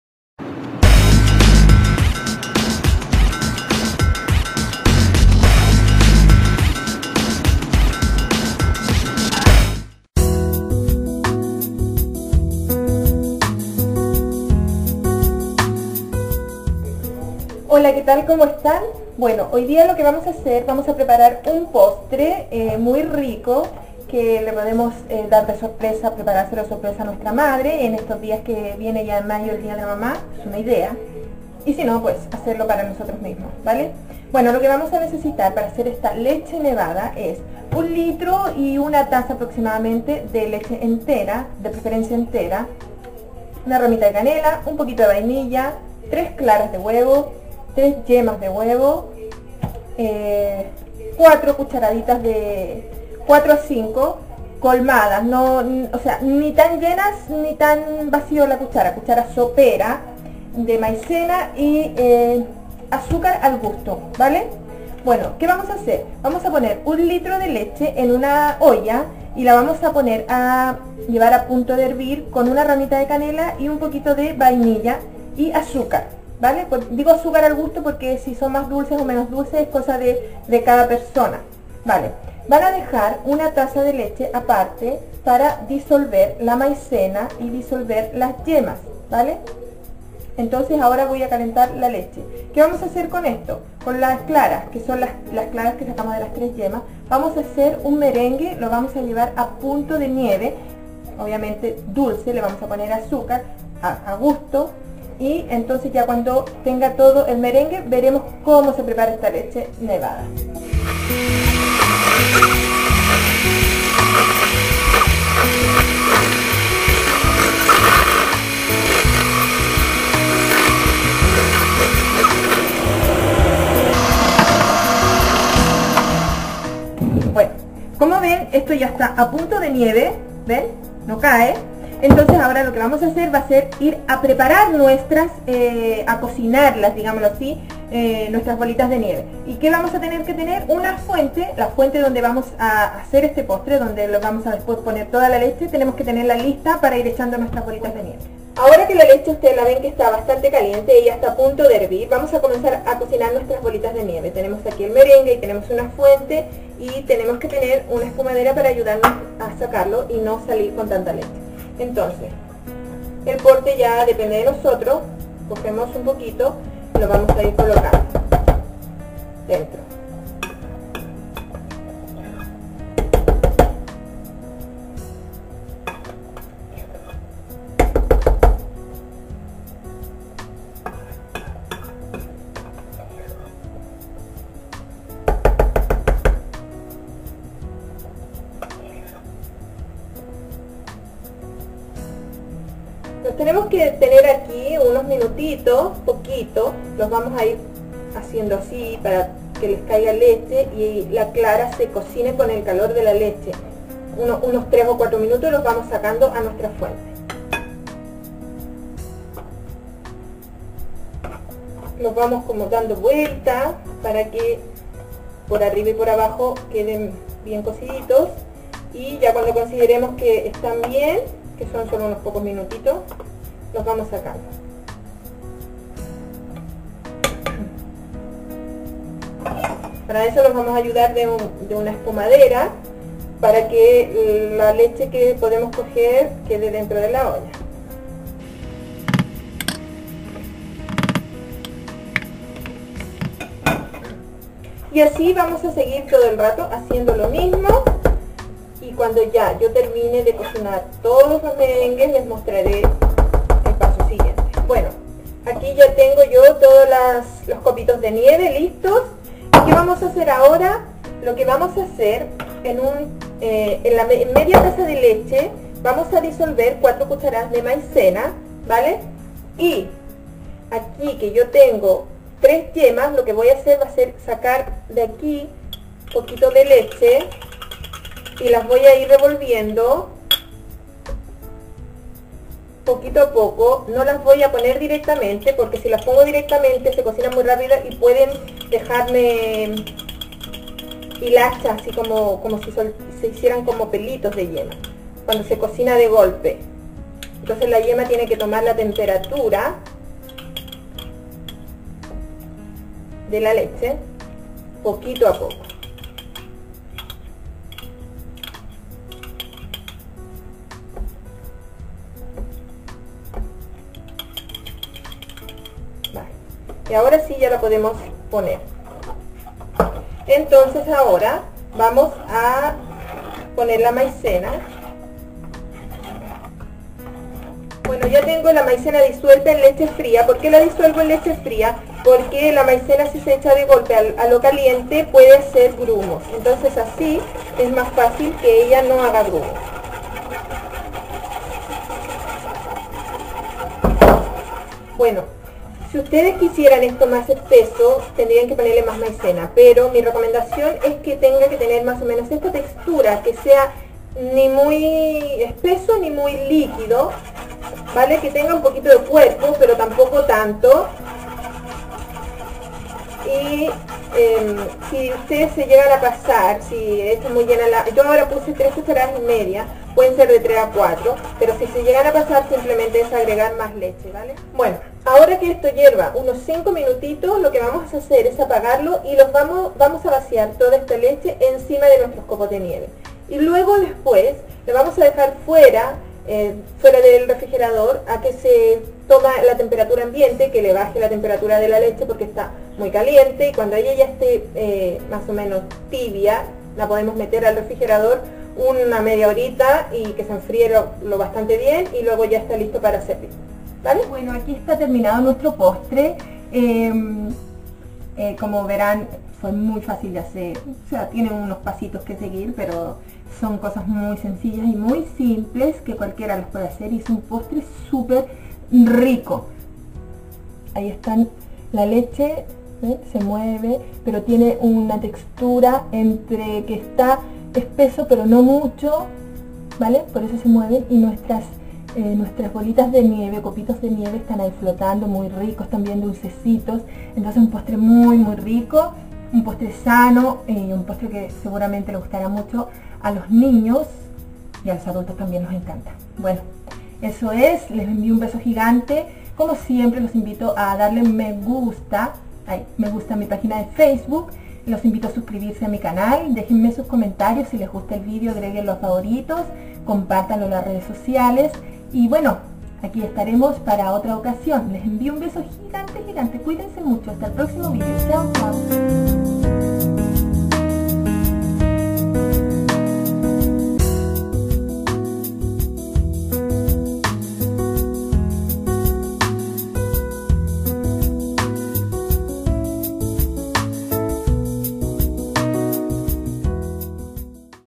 Receta: Postre "Leche Nevada" (Muy Rico Y Facil) - Silvana Cocina
En este video les mostrare paso a paso la receta para un delicioso postre de leche espesa con un toque de vainilla y canela con suaves copos de merengue :).